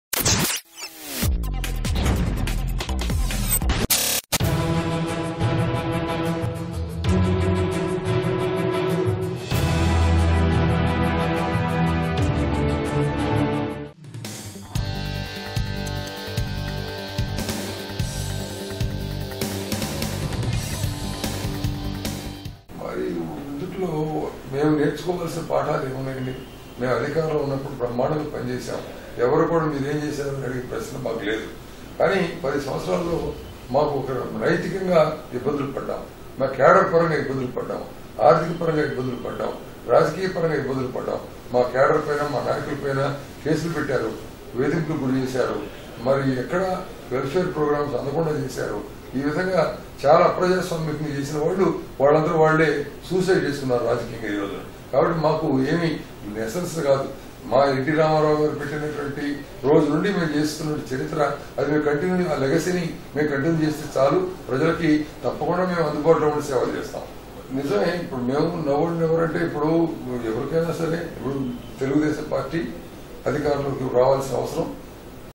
TDP will always continue the legacy left behind by NTR - Kodela Siva Prasad Rao
Watch senior TDP leader Dr Kodela Siva Prasad Rao speak about his experience when TDP was not in power.